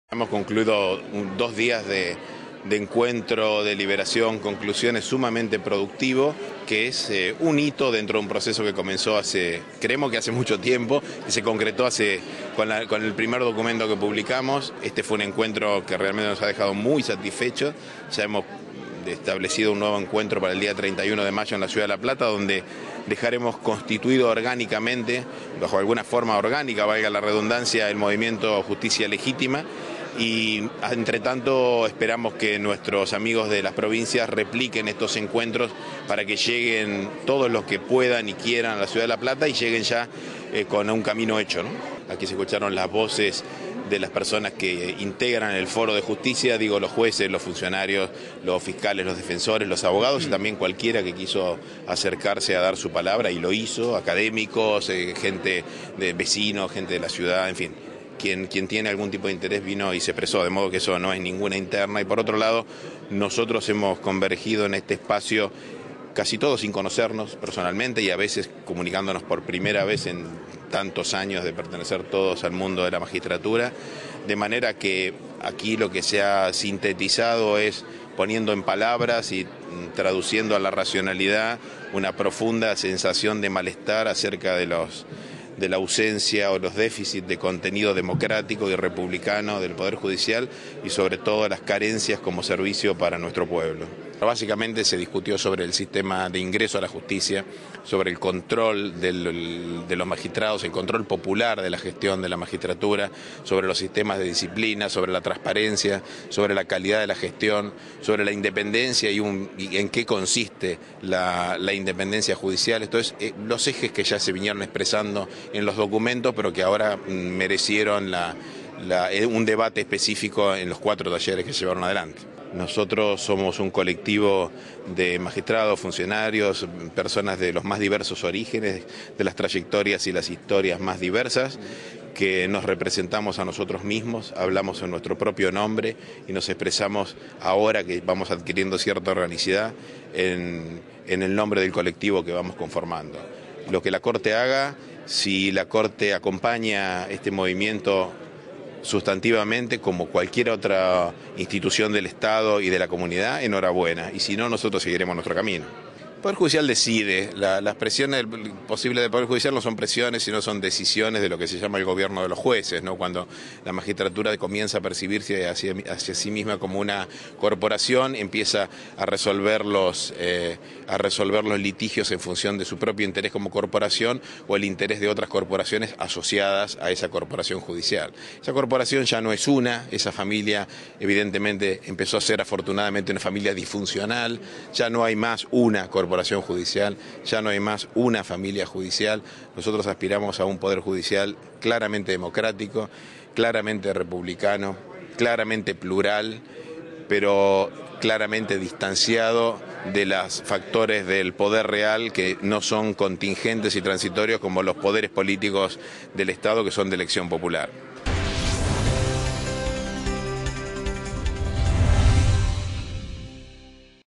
El movimiento "Justicia Legítima" se constituirá orgánicamente como asociación
Los encuentros por una Justicia Legítima celebrados ayer y hoy en la Biblioteca Nacional
tendrán su réplica el próximo 31 de mayo en la Ciudad de La Plata, donde quedarán "constituidos orgánicamente"
como asociación.El objetivo será el de profundizar aún más el debate sobre
cómo democratizar el sistema judicial, eje principal de los que
convocaron a más de dos mil asistentes en la Biblioteca Nacional.
La noticia fue confirmada por el fiscal federal Félix Crous,
uno de los organizadores de las jornadas y quien fuera
recientemente nombrado como responsable de la nueva Procuraduría de
Narcocriminalidad (PROCUNAR), y que también bregó porque la
discusión se "replique en todas las provincias" del país.
"Hemos concluido dos días de encuentros, deliberaciones y
conclusiones sumamente productivas; esto es un hito dentro de un
proceso que comenzó hace mucho tiempo y se concretó con la
publicación de nuestro primer documento", celebró Crous, tras
finalizar la lectura de las conclusiones que arrojaron los cuatro
talleres realizados en el día de la fecha.
Por su parte, la Defensora General de la Nación, Stella Maris
Martínez, afirmó al realizar un balance en diálogo con la prensa,
que "hay puntos que han quedado claros como el ingreso irrestricto
al sistema judicial mediante examen, la transparencia, la necesidad
de participación ciudadana, de jueces que sean realmente
independientes de todos los factores de poder y que se nutra del
apoyo del pueblo para tener esa independencia".
"Nosotros no somos un grupo disidente. Formamos todos parte
del sistema de administración de justicia, y estamos
expresándonos", sostuvo.
Y agregó: "Acá no hay ningún grupo disidente, acá hay gente
que le parece que hay que cambiar las cosas en el Poder Judicial, y
que se cambió mucho, se hicieron cosas muy buenas, se avanzó, pero
creemos que hay que avanzar un poco más, y quizás en ciertos temas
hacerlo más rápido"